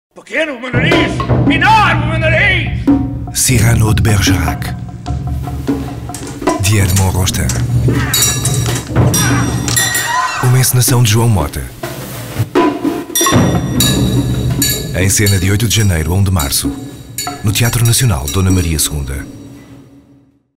Cyrano de Bergerac
SALA GARRETT - 8 JAN A 1 MAR 2015

Um apaixonado poeta. Um perspicaz dramaturgo. Um exímio espadachim. Um bravo soldado. Um grande filósofo. Um profundo estudioso da Física, Matemática e Astronomia. Cyrano de Bergerac possui qualidades incomuns, porém encobertas pelo aspeto físico, onde o nariz avantajado é motivo de frustração. Na França do século XVII, Cyrano sofre por amar intensamente sua prima, Roxanne, jovem, bela, emotiva, que tem como ideal de homem a beleza e o espírito. Ao conhecer Christian, Roxanne apaixona-se por ele, mas este é tímido e não consegue manter uma relação normal com uma mulher. É então que Cyrano ajuda Christian, escrevendo-lhe longas e belas cartas de amor que vão tornar ainda maior a paixão de Roxanne por Christian.

Esta é uma comédia heróica que exalta sentimentos nobres como o amor e a capacidade de renúncia. Escrita em 1897 por Edmond Rostand, baseada na vida de Cyrano de Bergerac, escritor francês, esta peça é considerada o último grande mito do teatro romântico francês. Na senda aberta por Ruy Blas, de Victor Hugo, e por Os Três Mosqueteiros, de Alexandre Dumas, Rostand criou com Cyrano de Bergerac uma peça única da dramaturgia, escrita em forma de poema.

de Edmond Rostand
tradução Nuno Júdice
adaptação João Maria André
versão cénica e encenação João Mota
cenografia José Manuel Castanheira
figurinos Storytailors
desenho de luz P. Graça
música Pedro Carneiro
desenho de som e sonoplastia Pedro Costa, Rui Dâmaso, Sérgio Henriques
mestre de esgrima Miguel de Andrade Gomes
caracterização Nuno Elias
cabelos Helena Vaz Pereira para Griffe Hair Style
maquilhagem Carla Pinho
assistentes de encenação Fábio Vaz (estagiário da ESTC)
assistente de cenografia Luna Rebelo (estagiária)
produção TNDM II

duração 2h25 (com intervalo)
M/12

Nota: As cortinas do palco cenográfico no 1º acto, contêm elementos decorativos inspirados na Capela de Santa Comba, Telões/Baião. Alguns outros telões cénicos contêm referências à pintura de John Constable (1776 – 1837) e de Claude Lorrain (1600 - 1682).

©Pedro Macedo - Framed Films